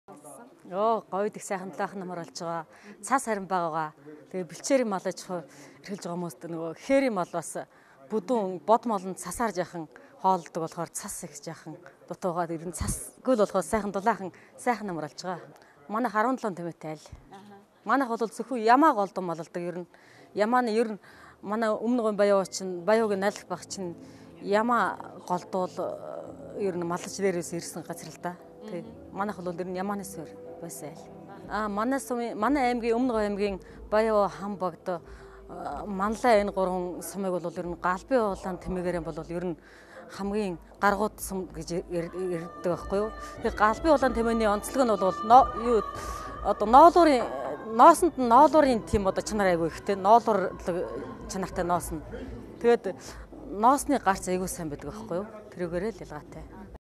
Малчин Б.Бямбатөмөр: Ханбогд, Манлай, Баян-Овоо сумдууд Галбын улаан тэмээгээрээ алдартай
Оюу Толгой компанийн Бүсийн хөгжил нийгмийн хэлтсийн хэрэгжүүлдэг "Орон нутгийн бизнес эдийн засгийг дэмжих" хөтөлбөрийн хүрээнд тэмээний ноосон бүтээгдэхүүний брэнд бий болгох санаачилгыг энэ жилээс өрнүүлжээ. Энэ санаачилгын хүрээнд тэмээний ноосийг эцсийн бүтээгдэхүүн болгох нэмүү өртөг шингээх шат дамжлагуудыг тодорхойлж, улмаар энэхүү өртгийн сулжээнд оролцож буй талуудын харилцан хамаарлыг судалжээ. Эдгээр уялдаа холбоог сайжруулснаар тэмээний ноосон бүтээгдэхүүний зах зээл нэмэгдэж, үндэсний үйлдвэрлэгчид бэхжиж улмаар малчдын амьжиргааг тогтвортойгоор сайжруулах боломж бүрдэх юм.